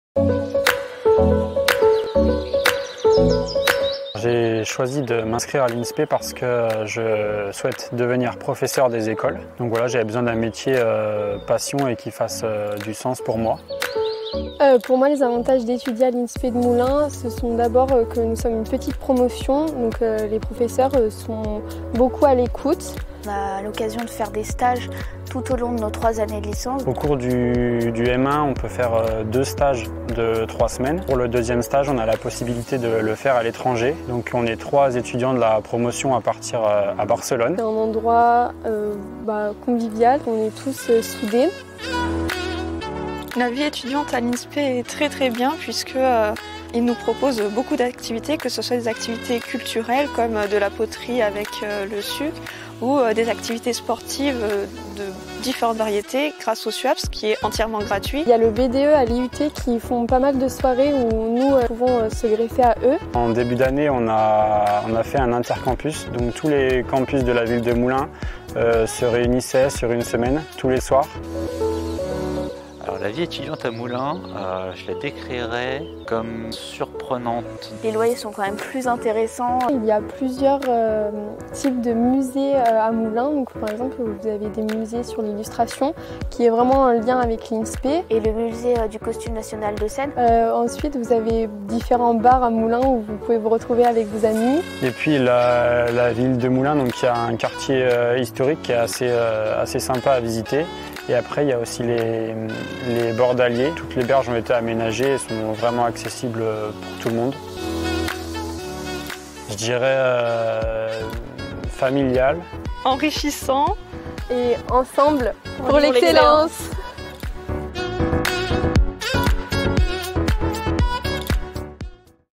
La vie universitaire à l'INSPE Moulins
Envie de devenir professeur des écoles ?

As-tu pensé au campus de Moulins de l’INSPE Clermont-Auvergne ?

Proximité avec les formateurs, culture, sport, histoire, coût de la vie...Il y a tant de raisons d'étudier dans l’Allier!

Pour en savoir plus sur le Master MEEF 1er degré de l'INSPE Clermont-Auvergne, site de Moulins

Retrouvez aussi la Licence SDEF niveau 1, 2 et 3, parcours PPPE sur le site de Moulins :